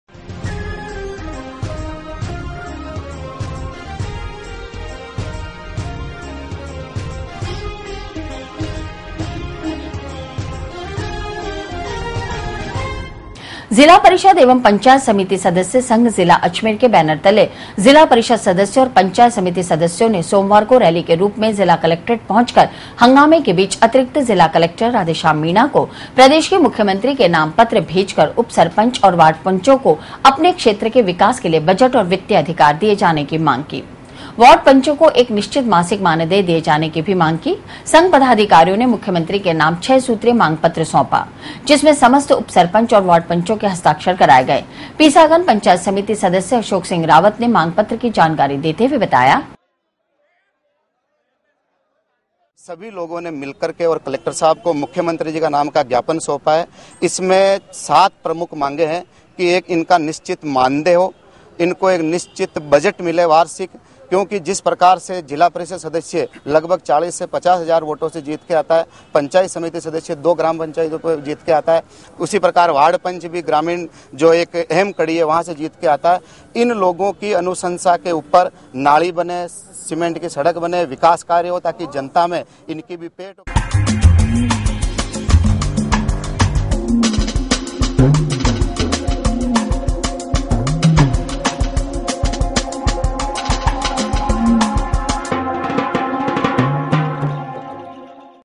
जिला परिषद् और पंचायत समिति सदस्यों ने मांगा विकास बजट
जिला परिषद् और पंचायत समिति सदस्यों ने विकास बजट और वित्तीय अधिकार के साथ मासिक मानदेय देने की उठाई मांग। रैली निकालकर कलेक्टर को मुख्यमंत्री के नाम दिया ज्ञापन।